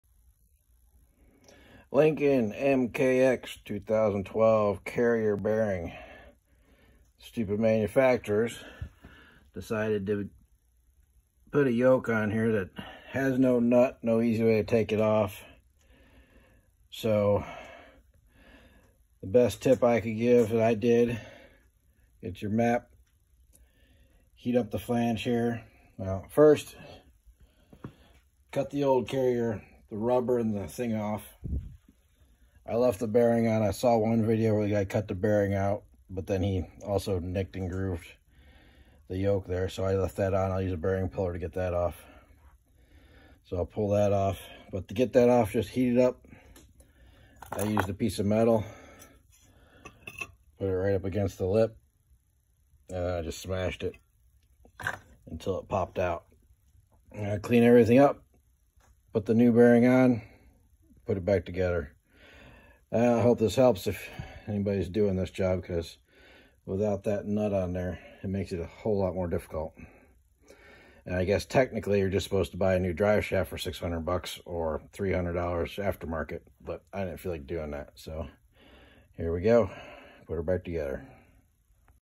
2012 Lincoln MKX carrier bearings
Lincoln MKX 2012, drive shaft carrier bearings replacement. How to separate yolk from shaft.